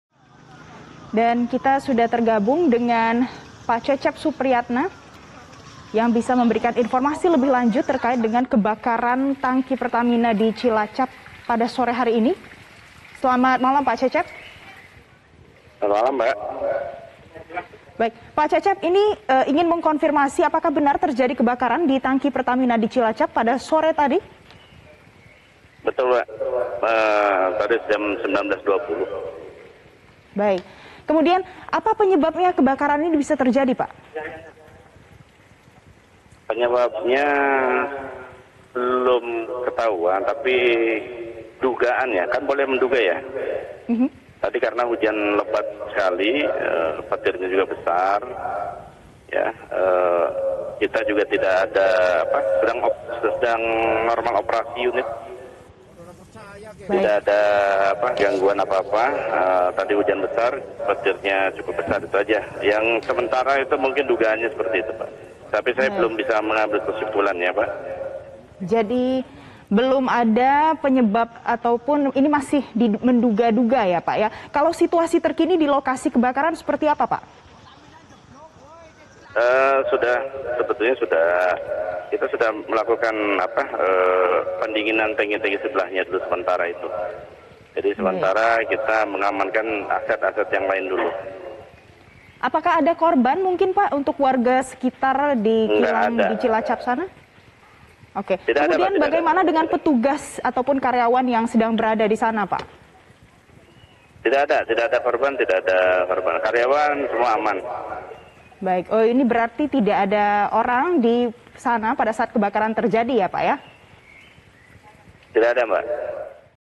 Tangki Pertamina di Cilacap Terbakar Diduga Akibat Tersambar Petir
Tangki Kilang Pertamina RU IV Cilacap, Jawa Tengah dikabarkan terbakar pada Sabtu (13/11/2021) sekitar pukul 19.00 WIB. Informasi tersebut pertama kali tersebar melalui aplikasi pesan singkat Whatsapp. Area Manager Pertamina Unit Cilacap Cecep Supriyatna menyebut dugaan sementara kebakaran disebabkan oleh sambaran petir.